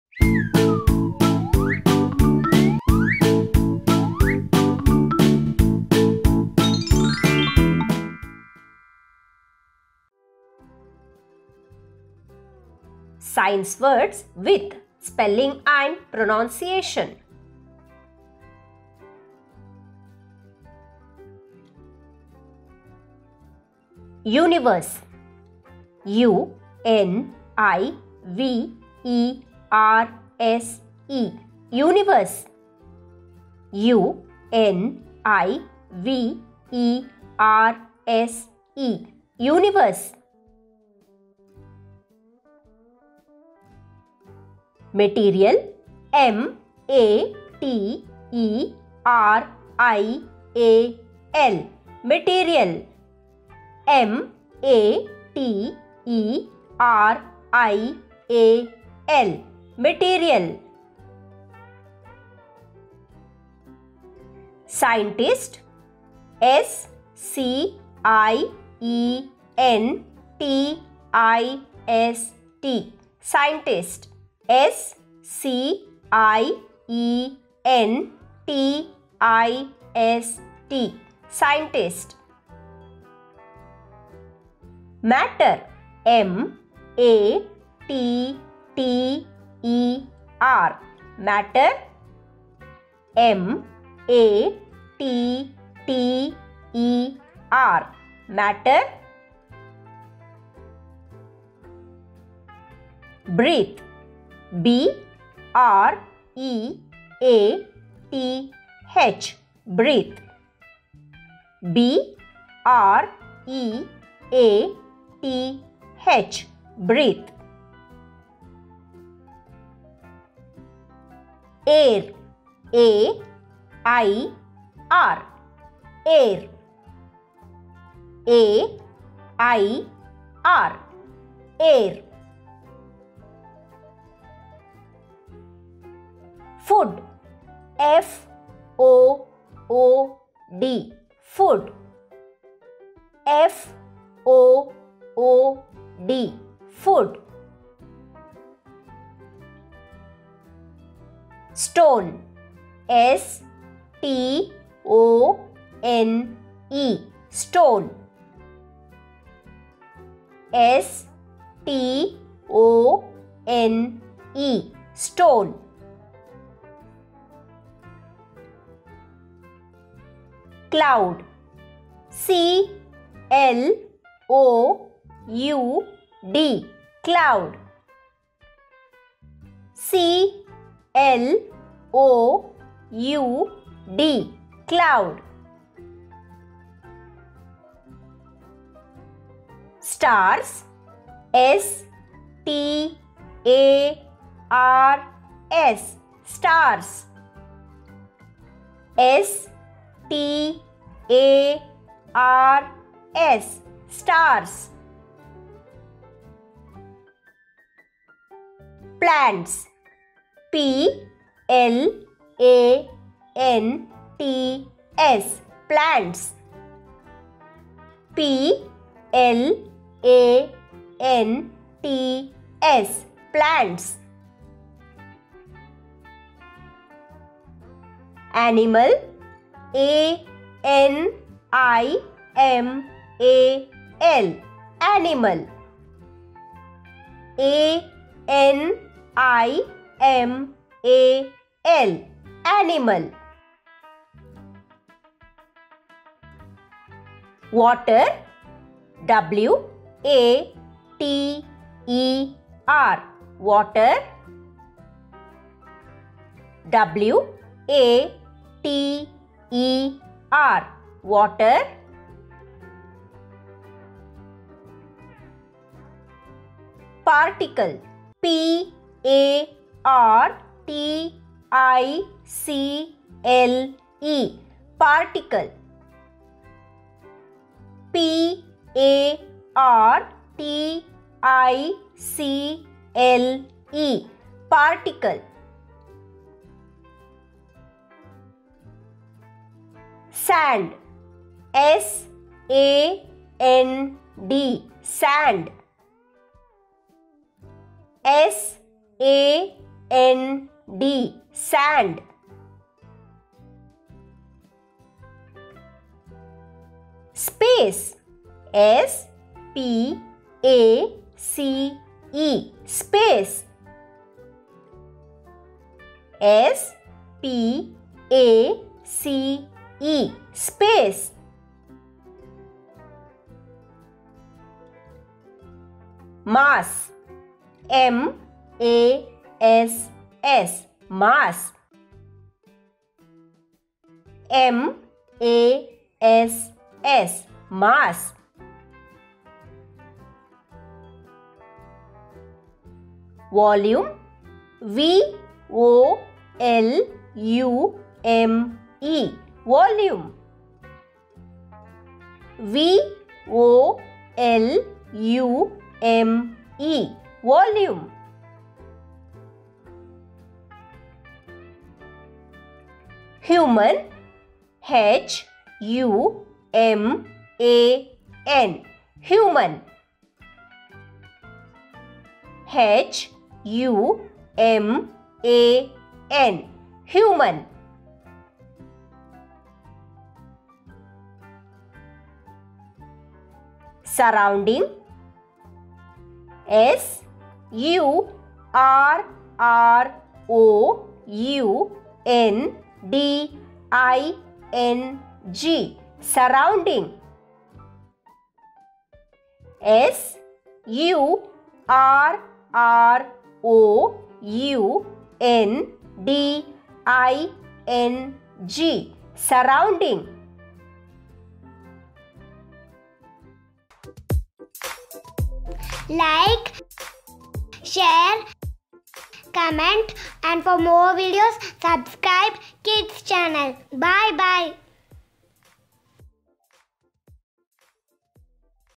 Science words spelling and pronunciation-3 | Science words for kids | Kids Channel
Science words for kids to learn spellings and pronunciation.
This video is for kids to learn Science words with fun. You can plug and watch in TV and ask your kids to repeat..

To learn more about activities join our kids channel group.